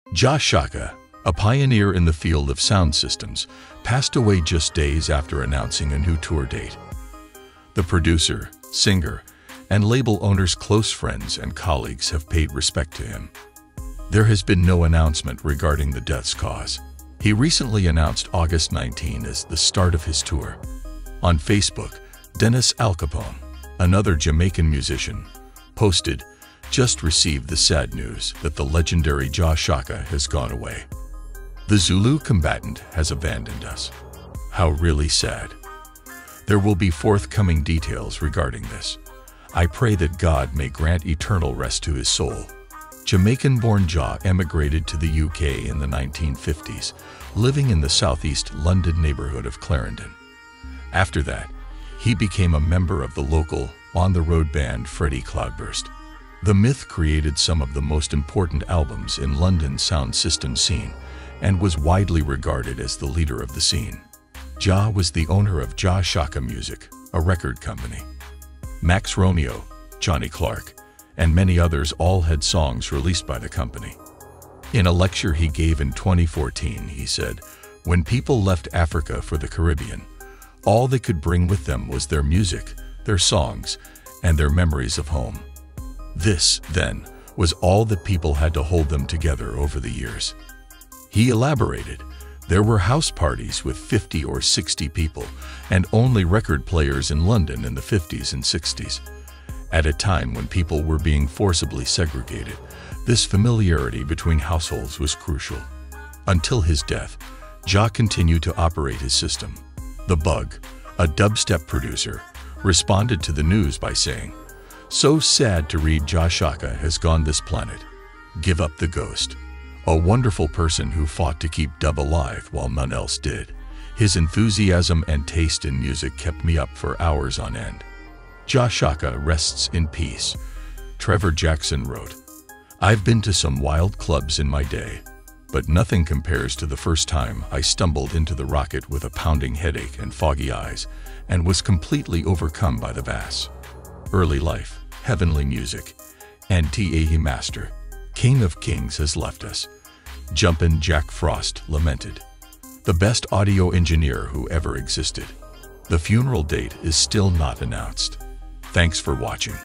RIP Funeral of Jah Shaka, Dub and Roots Reggae Legend Who died Recently
Josh Shaka a Pioneer in the field of sound systems passed away just days after announcing a new tour date the producer singer and label owner's close friends and colleagues have paid respect to him there has been no announcement regarding the death's cause he recently announced august 19 as the start of his Tour on Facebook Dennis Al Capone another Jamaican musician posted just received the sad news that the legendary jaw Shaka has gone away the Zulu combatant has abandoned us how really sad there will be forthcoming details regarding this I pray that God May Grant Eternal rest to his soul jamaican-born jaw emigrated to the UK in the 1950s living in the Southeast London neighborhood of Clarendon after that he became a member of the local on the road band Freddie cloudburst the myth created some of the most important albums in London's sound system scene and was widely regarded as the leader of the scene jaw was the owner of jaw Shaka music a record company Max Romeo Johnny Clark and many others all had songs released by the company in a lecture he gave in 2014 he said when people left Africa for the Caribbean all they could bring with them was their music their songs and their memories of home this then was all that people had to hold them together over the years he elaborated there were House parties with 50 or 60 people and only record players in London in the 50s and 60s at a time when people were being forcibly segregated this familiarity between households was crucial until his death Jacques continued to operate his system the bug a DubStep producer responded to the news by saying so sad to read Josh Shaka has gone this planet give up the ghost a wonderful person who fought to keep Dove alive while none else did his enthusiasm and taste in music kept me up for hours on end Josh Shaka rests in peace Trevor Jackson wrote I've been to some wild clubs in my day but nothing compares to the first time I stumbled into the rocket with a pounding headache and foggy eyes and was completely overcome by the bass early life Heavenly music and T.A he Master King of Kings has left us jump in Jack Frost lamented the best audio engineer who ever existed the funeral date is still not announced thanks for watching